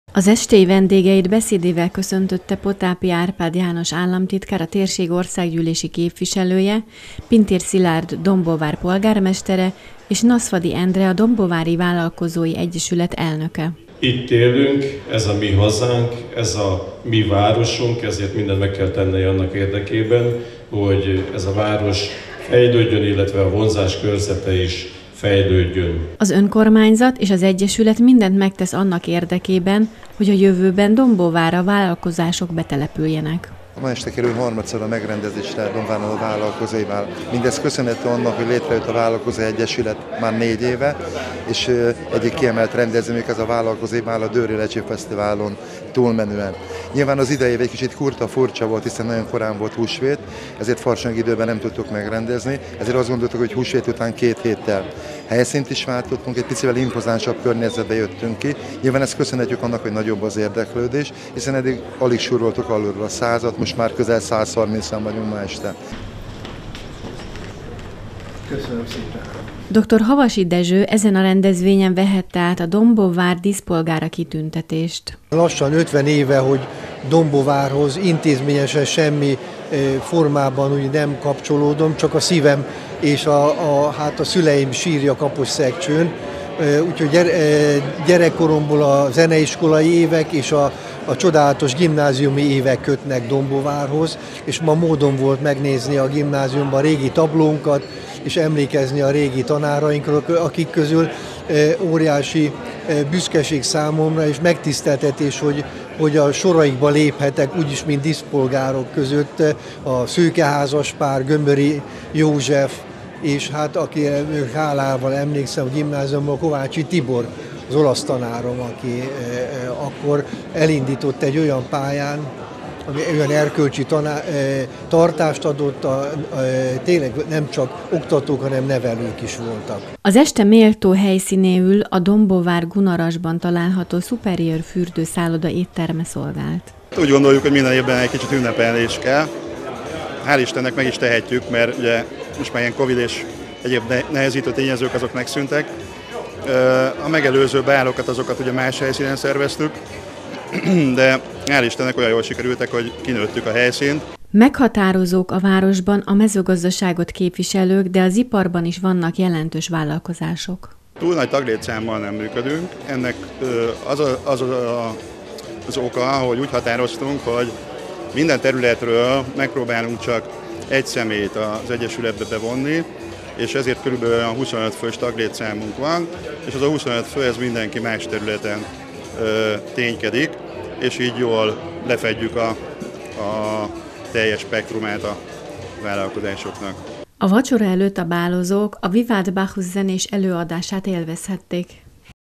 Dombóvár - Harmadik alkalommal báloztak a vállalkozók
Dombóvár vállalkozói báloztak. A nívós eseményt idén rendezték meg harmadik alkalommal, de a nagy érdeklődésre való tekintettel már új helyszínen, a Gunaras Resort SPA Hotelben.